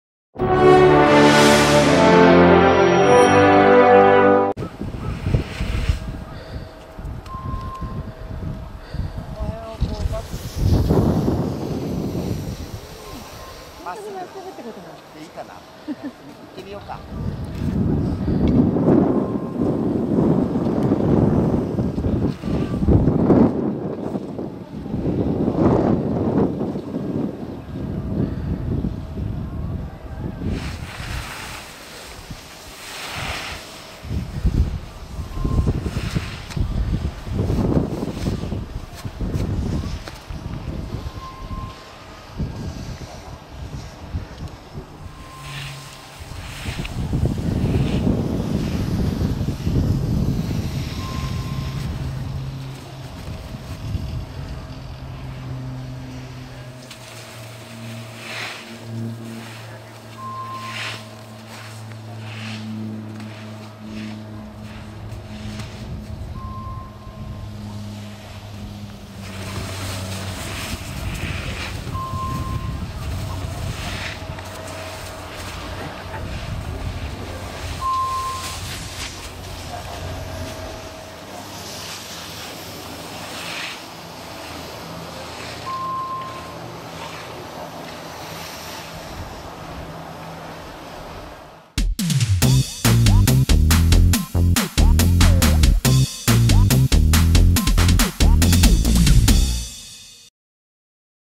佐藤美智夫2015（22）〜メイトレッスン・コブ〜
2015/03/22、湯殿山スキー＆ボードスクール佐藤美智夫校長のメイトレッスンを撮影しました。